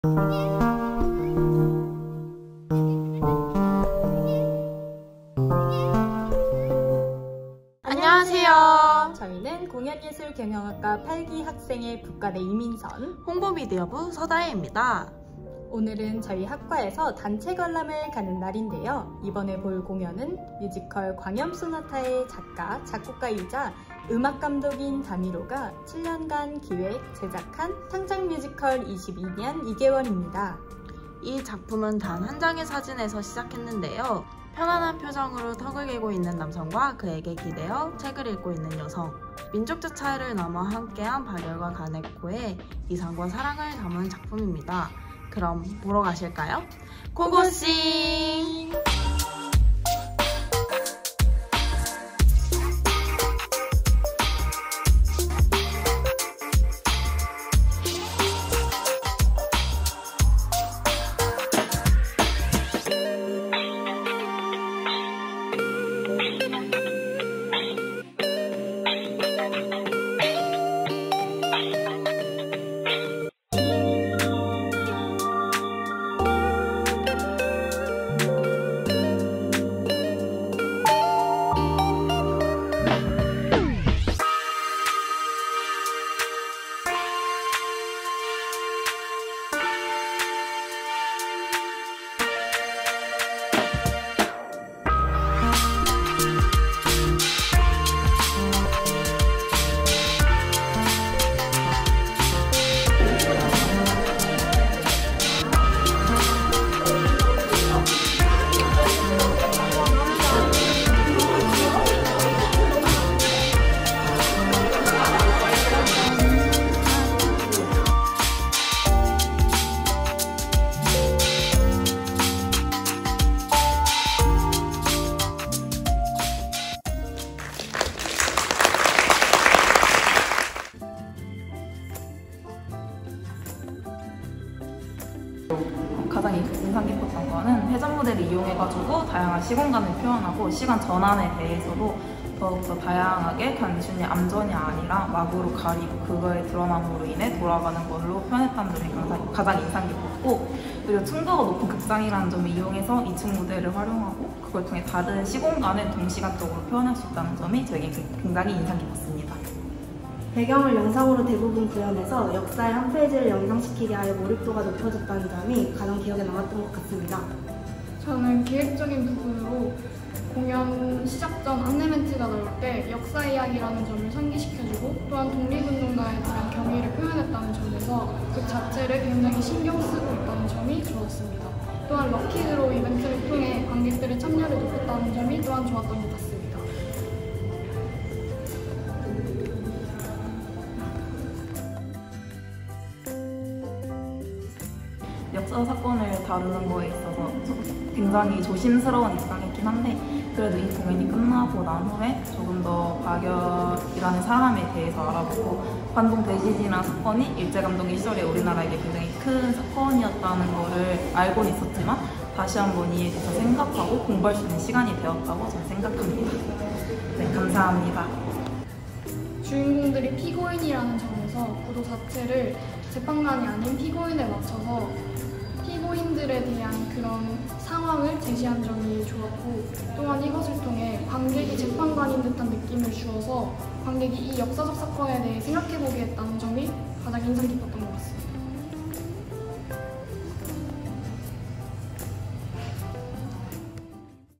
[VLOG] 백석예술대학교 공연예술경영과 l “22년 2개월” 단체관람 브이로그
2023년 11월 3일 금요일 백석예술대학교 공연예술경영과 뮤지컬 단체관람